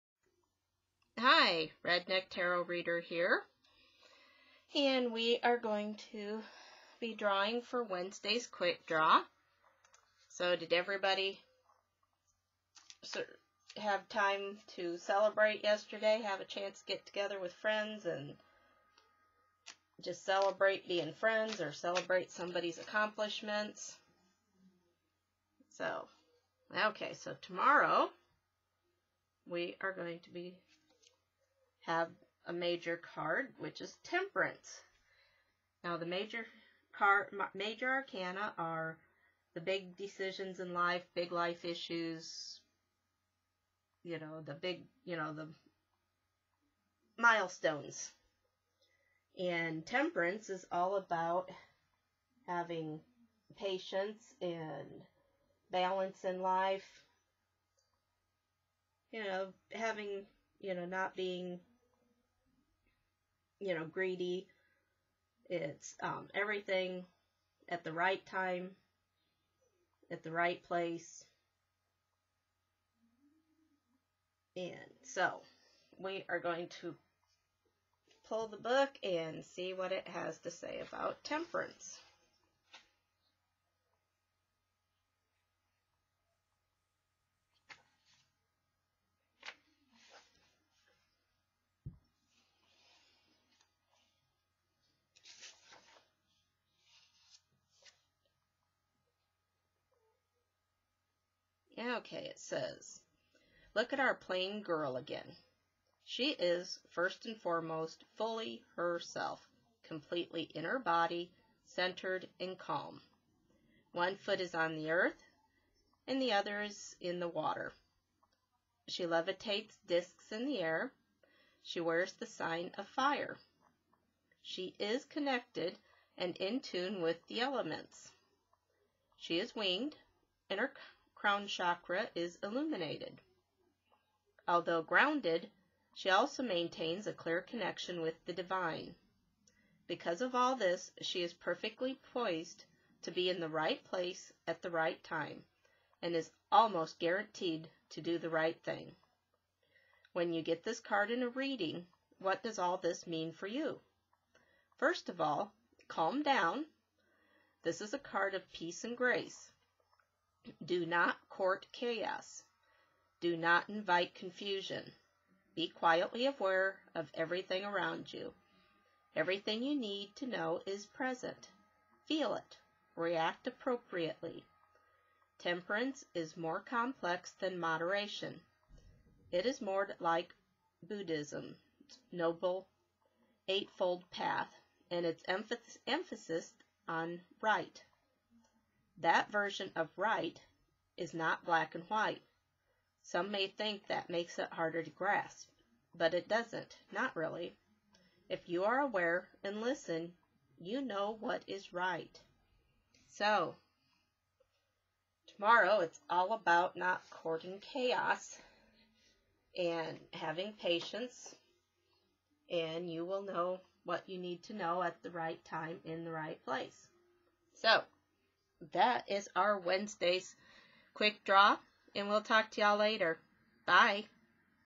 Tarot Quick Draw for 4 8 15
Wednesday's Tarot Quick Draw, Temperance, tells us we need to be patient, every thing will happen at the right time and the right place, and not to court chaos!